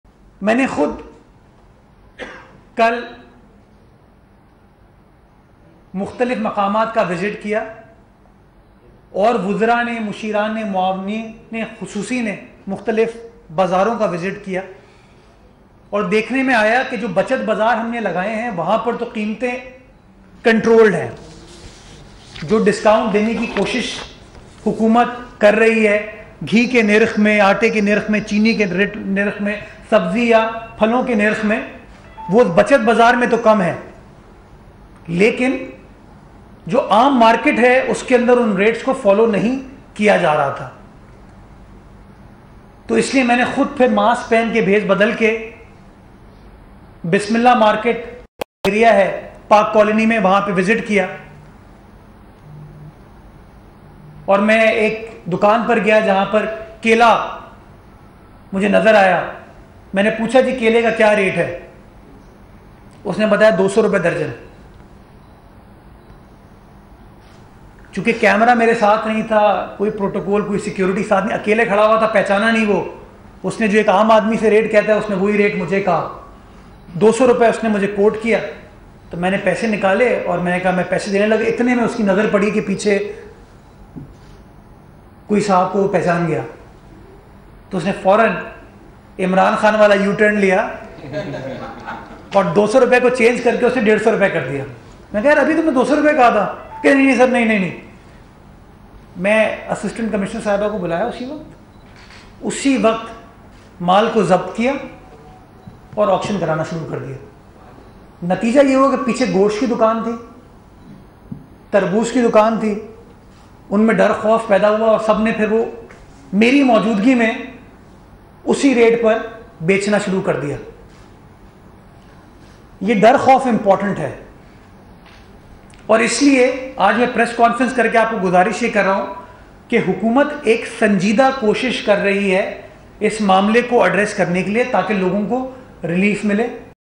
Murtaza Wahab Jo Action| Karachi Men Chaa Thiyo?
دڪاندارن ۾ حڪومت جو خوف پيدا ڪرڻ جي ڪوشش!
مرتضيٰ وهاب کي ٺڳيندڙ دڪاندار جو ڪهڙو حشر ٿيو؟